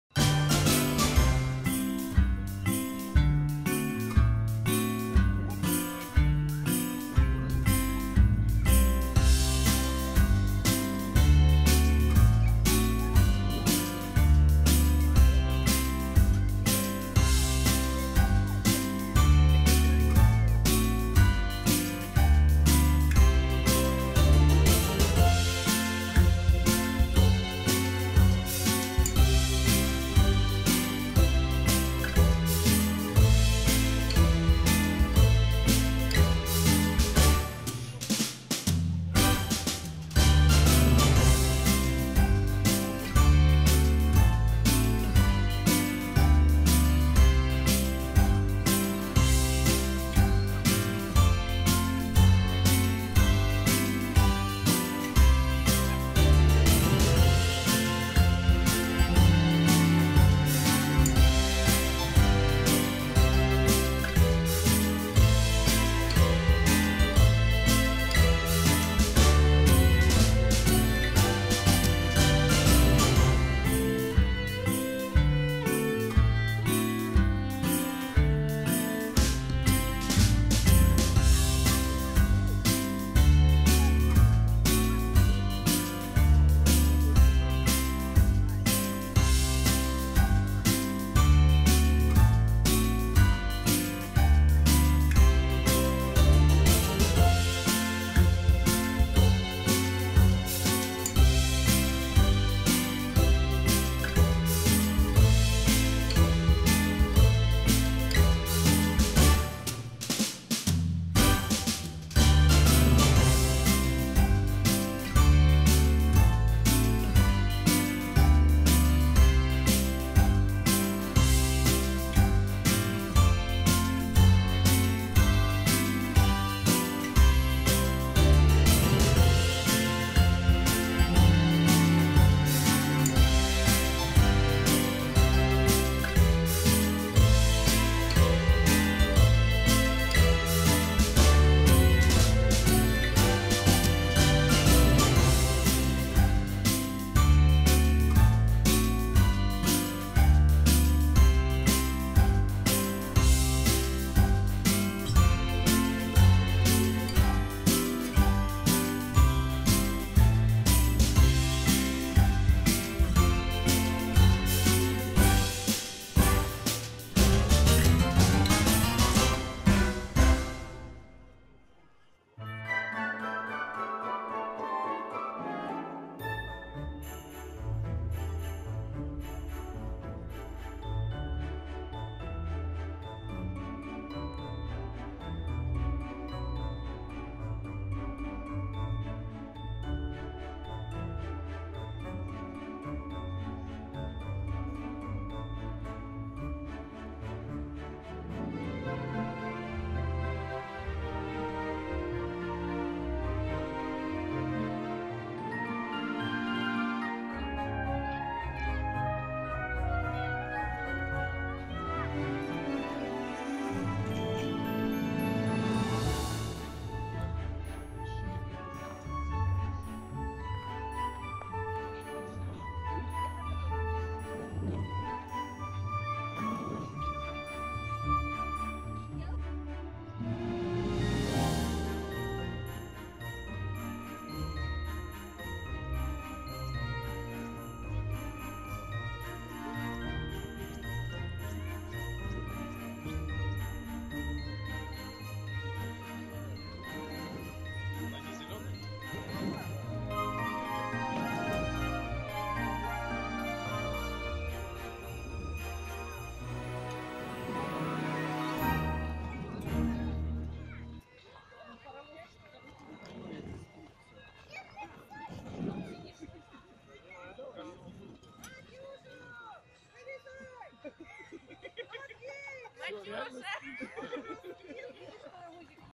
Новогодняя елка в Центральном парке Екатеринбурга
2019 год. Так где-то...
Центральный парк культуры имени Владимира Маяковского в Екатеринбурге. Предновогодние прогулки.
Если не быть ворчуном, можно неплохо прогуляться, не обращая внимания на перекрывающую перспективу новую будку по прокату вело на центральной аллее, не замечая старые шалманы и новые закусочные, которых мильон на квадратный метр. Можно даже закрыть глаза на цену билетов -  50 рублей за спуск с большой горки (других просто нет).